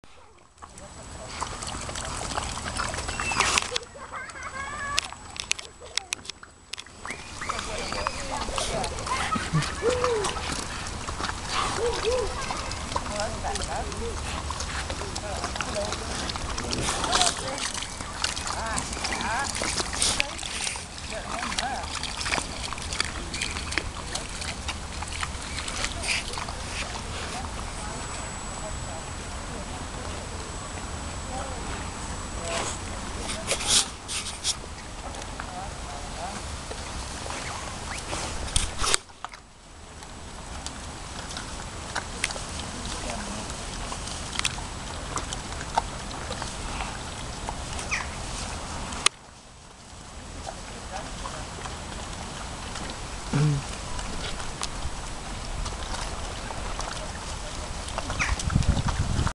Squelchy piggies at Quarr Abbey, Isle of Wight
Pigs walking around in mud at Quarr Abbey before it all got redeveloped. These were the good old days when we could stick our hands through the fence. Now it's double barriers.